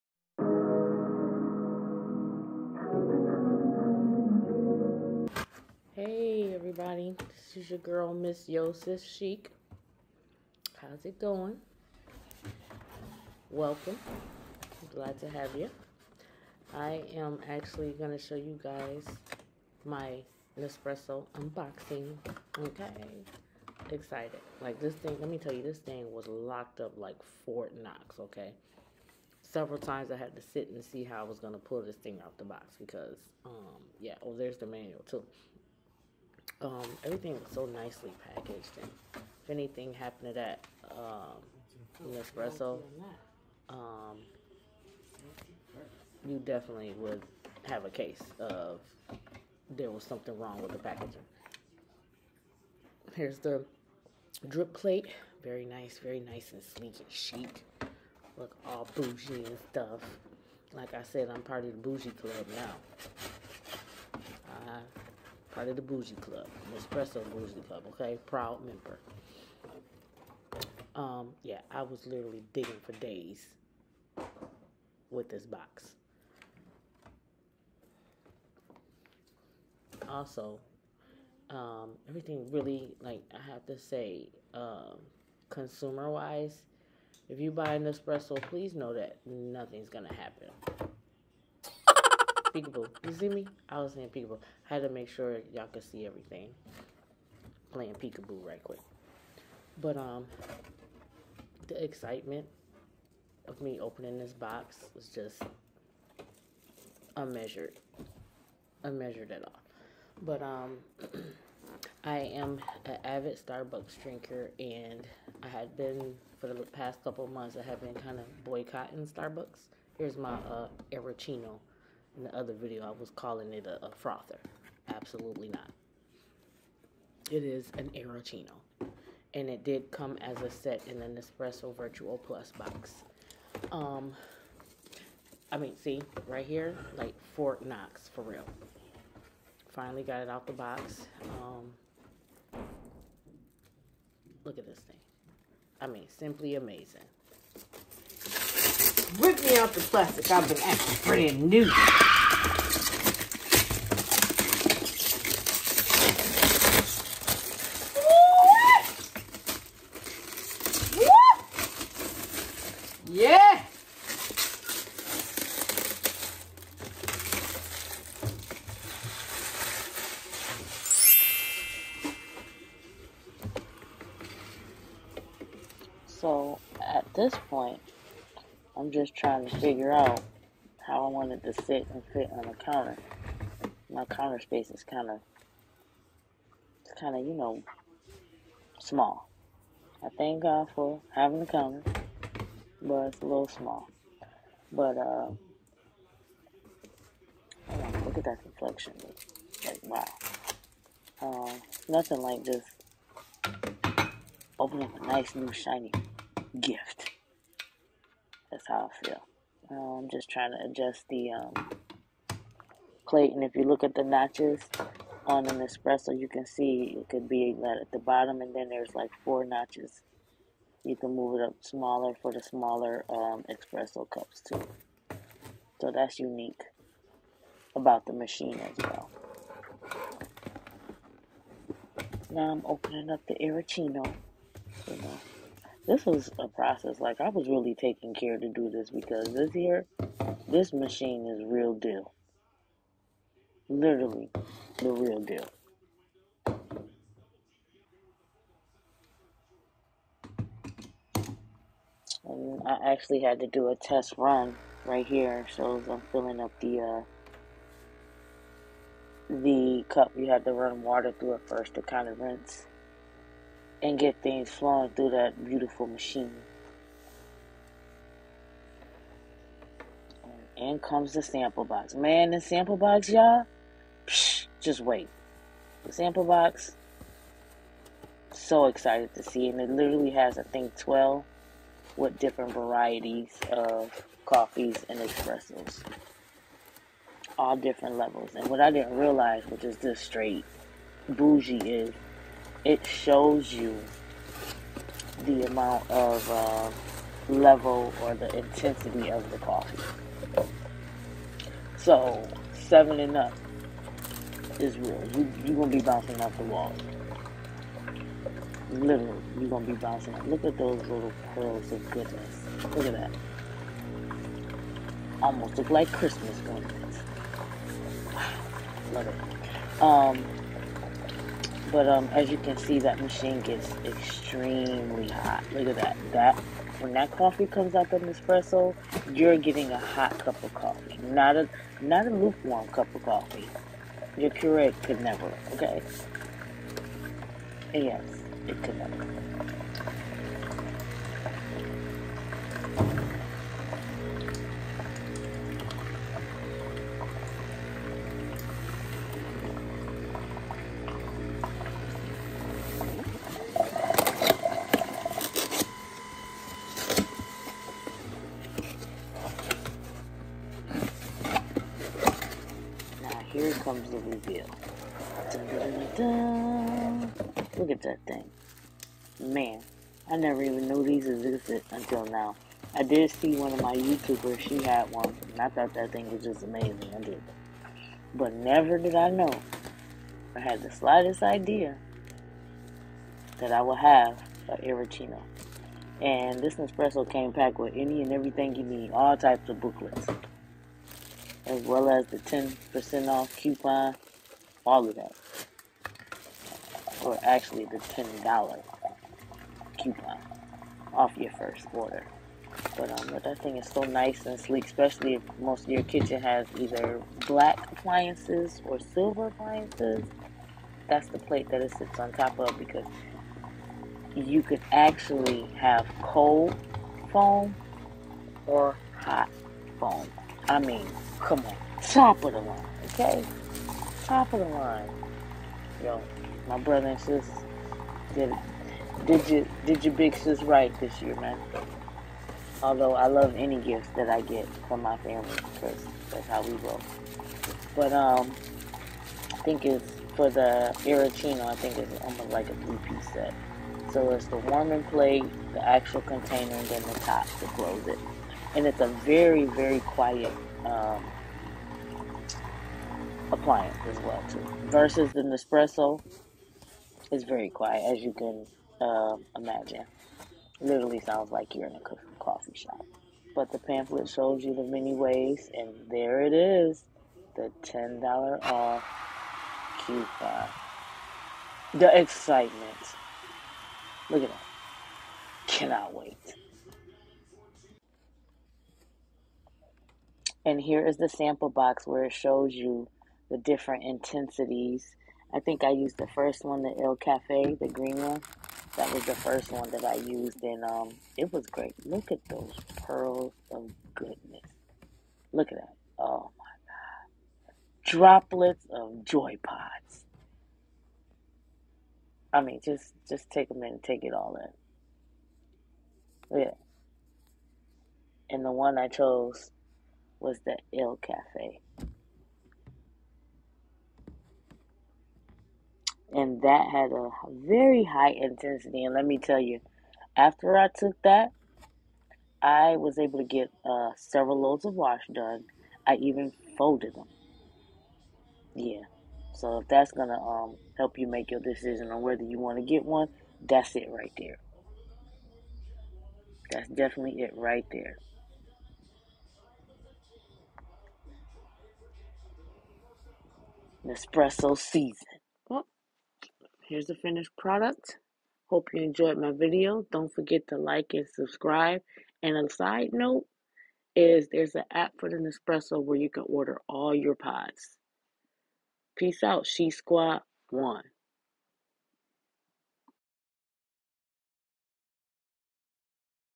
Nespresso Vertuo Plus Unboxing
I received a Nespresso Vertuo Plus for christmas this year and let me tell you that your "Other" coffee machine could never. Let me know below if you have one of these and what did you think about it. Share recipes for great drinks as well. Leave it in the comments below